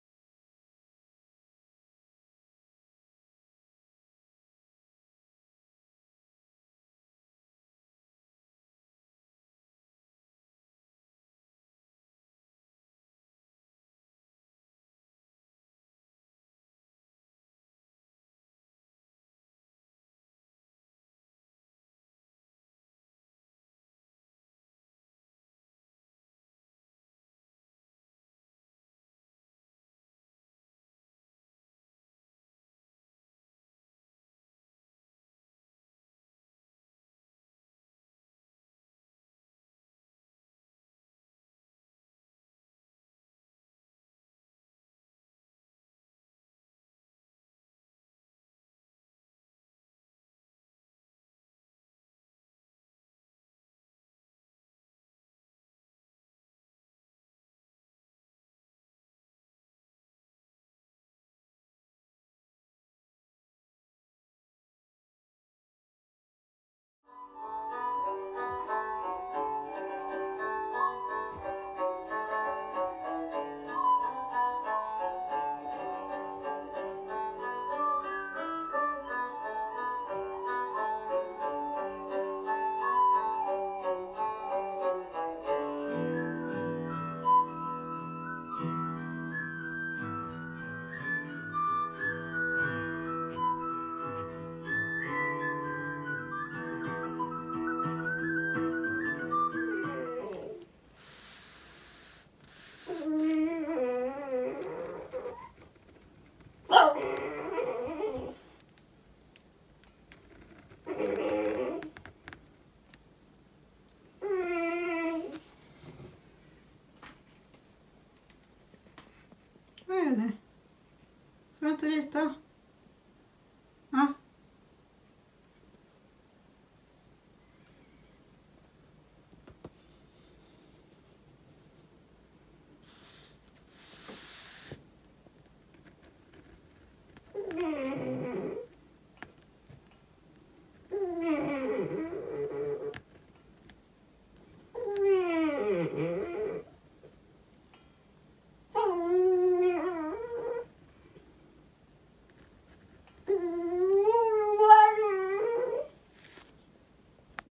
Lisette Knutsen - Art
A piece of work i have not touched for years... And now begins to come alive... Feels really great! And my dog Greta won’t let me draw...